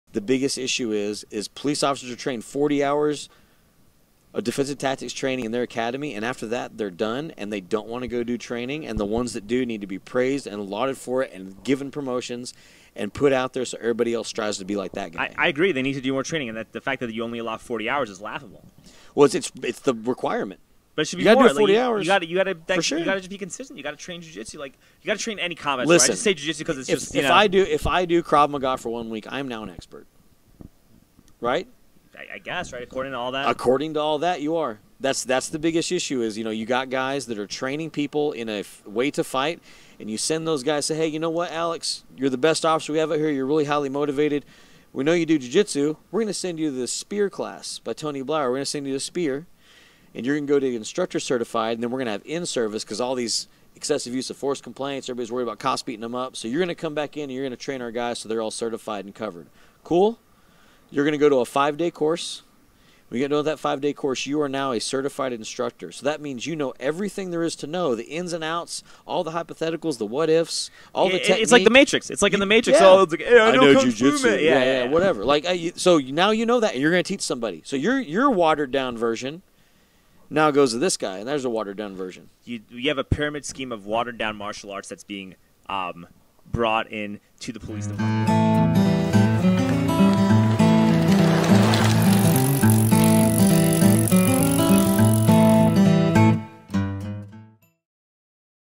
We’re Training Our Officers Wrong
Ex cop and BJJ Blackbelt Brad Barnes breaks down the problems with how our officers are physically trained.